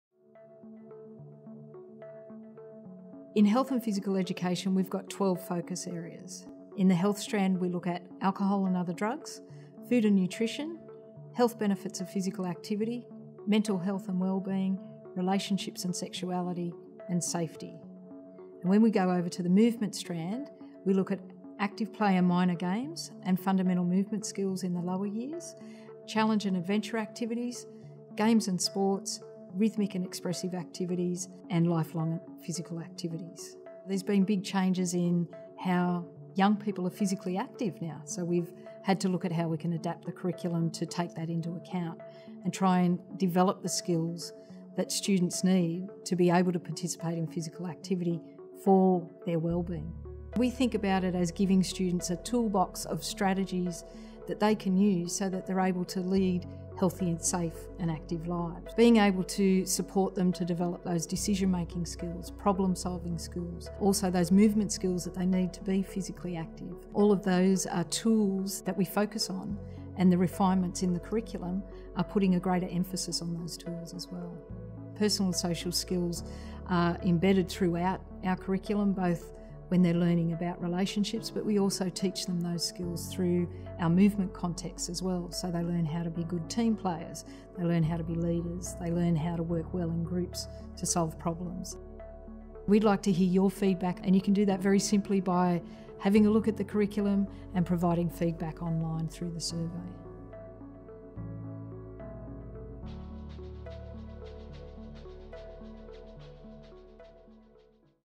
Talking the Australian Curriculum Review: HPE
ACARA's Janice Atkin discusses the Review of the Australian Curriculum: HPE.